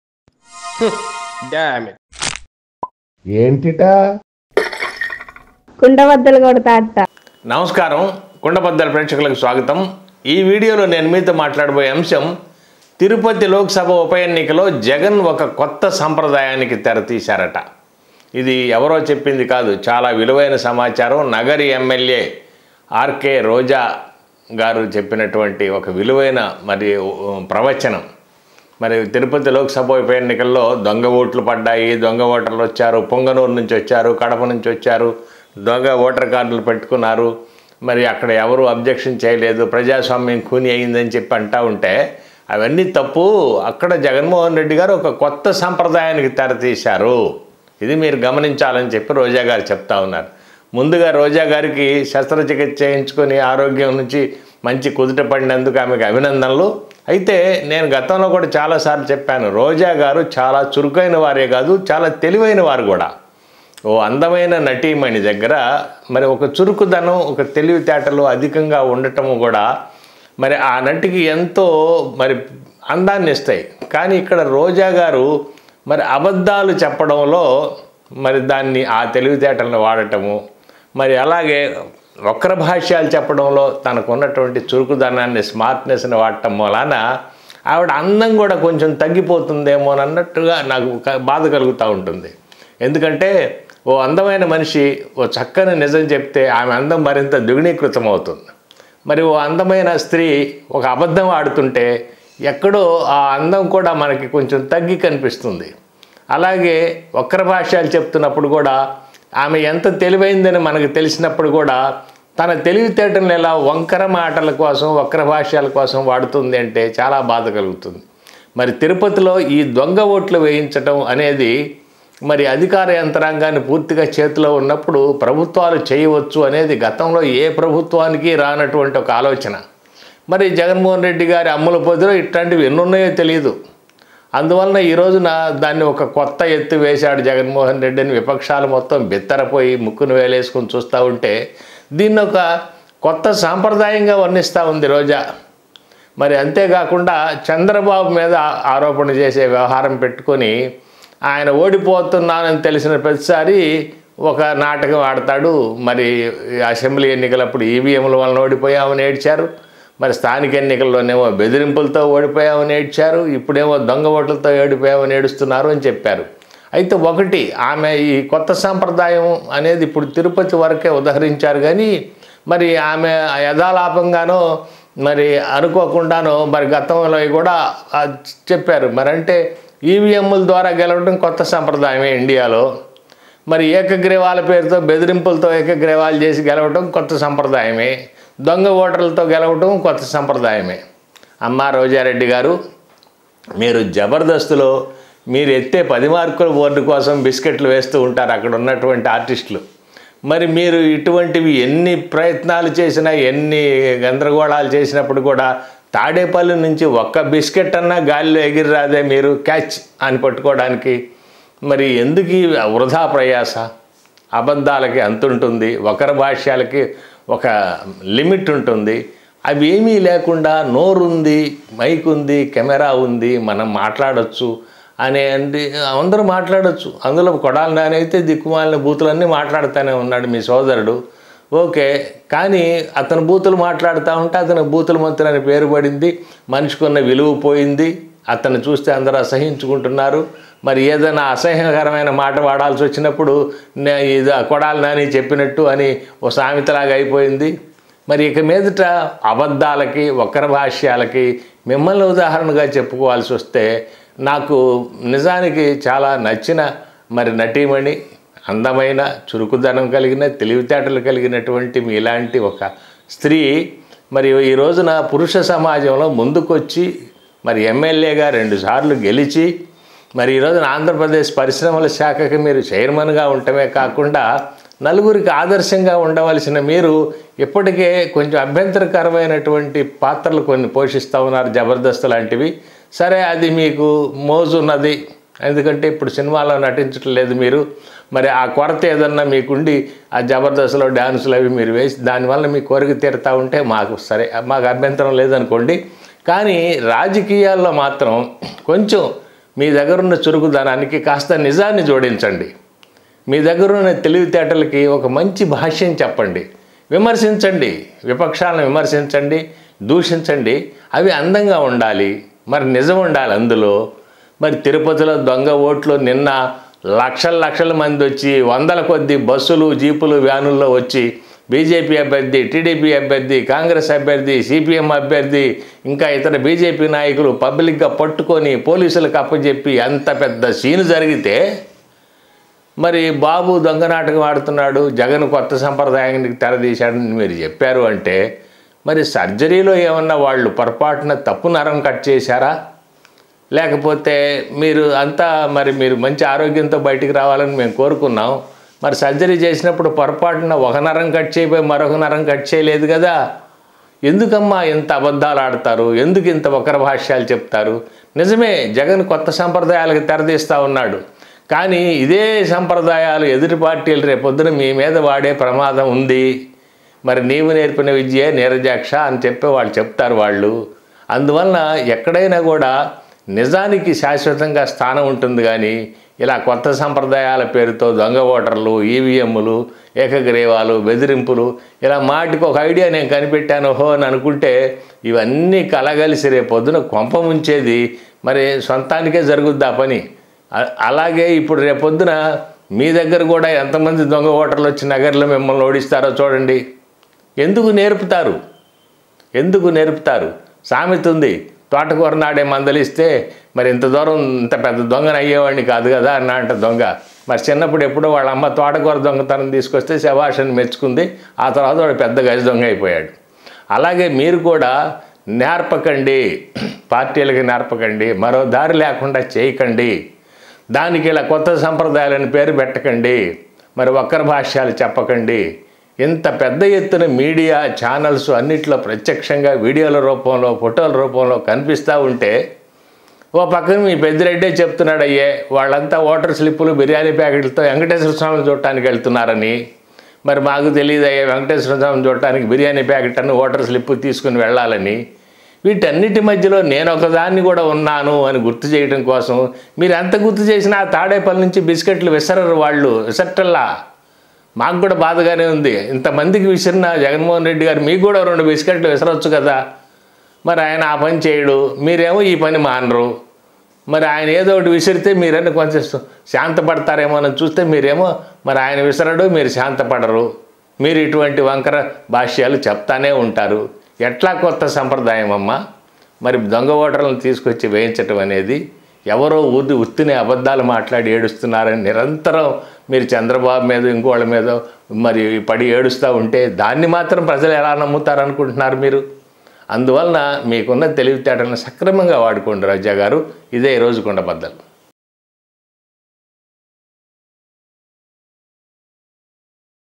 జగన్ కొత్త సంప్రదాయానికి తెరతీశాడంటున్న రోజా | EVMలు, ఏకగ్రీవాలు, దొంగ ఓటర్లు...ఆహా?
MLA Roja,
Kundabaddalu,